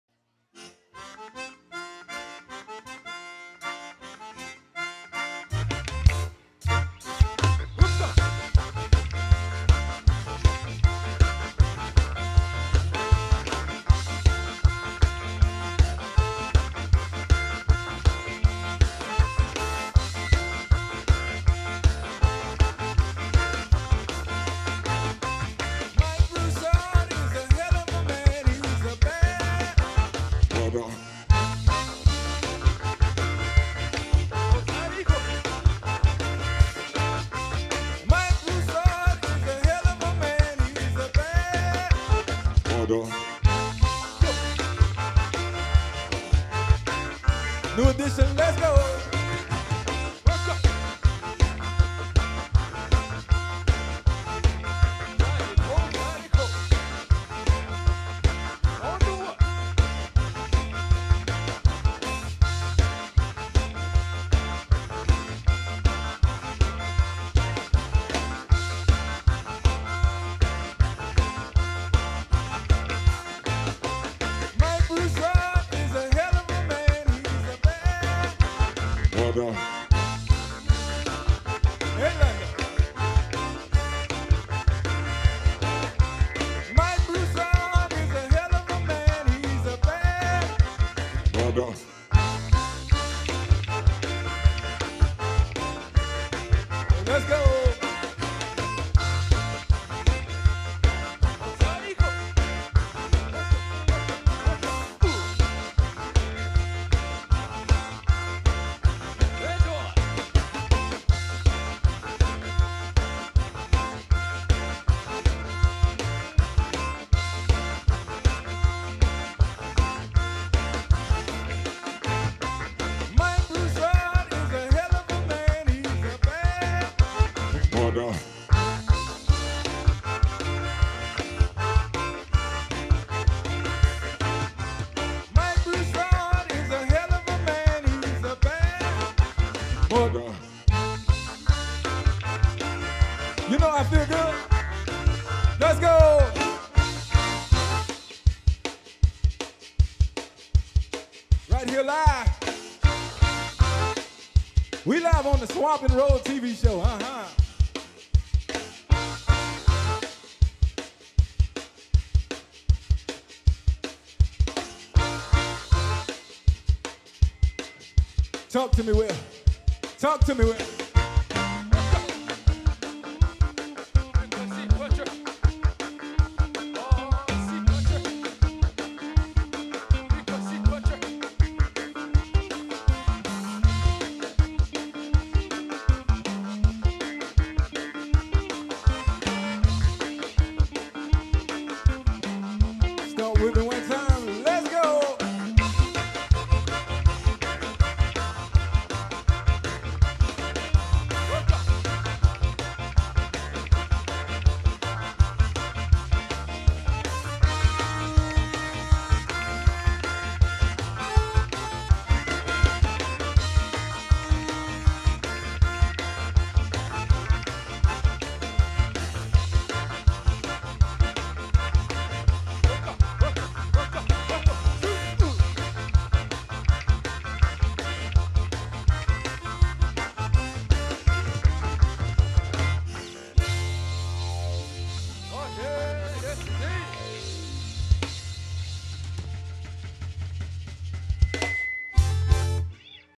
Mike Broussard - KDCG Song of the Day 07-24-19
Mike Broussard on the Swamp n Roll Show at Black Gold Casino!